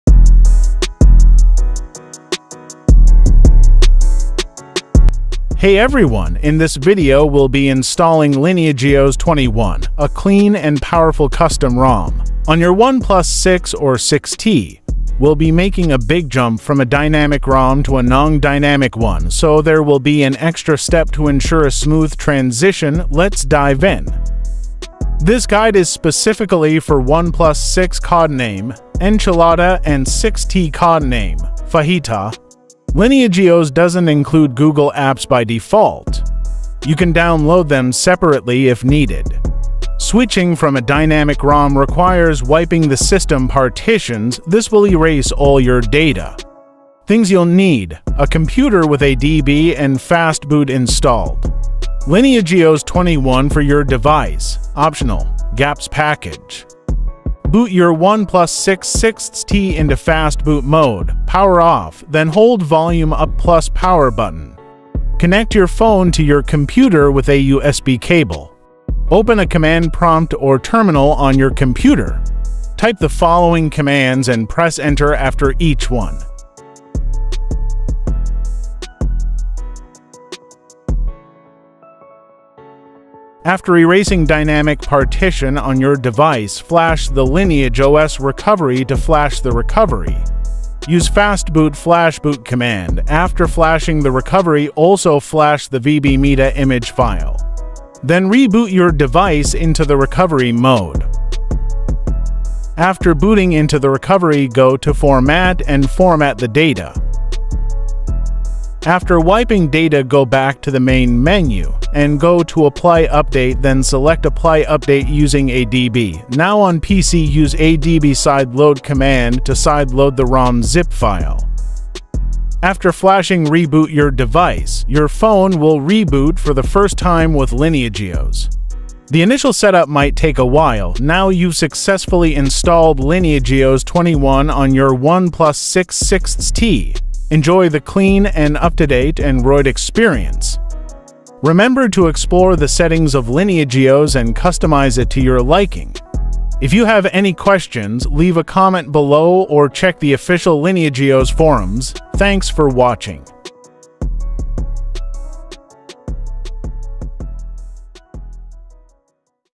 Install LineageOS 21 on OnePlus 6/6T (Dynamic to Non-Dynamic ROM)
Unleash the power of Android 14 on your OnePlus 6/6T with LineageOS 21! This video guides you through the entire process, including the crucial step of switching from a dynamic ROM to a non-dynamic one. We'll cover unlocking the bootloader, wiping for a clean install, and flashing LineageOS with optional GApps. Get ready for a smooth and speedy experience!

Note : This tutorial only shows how to flash over any Dynamic ROM. If you're coming from a non dynamic ROM then just flash the recovery(boot.img) and then sideload the ROM zip file.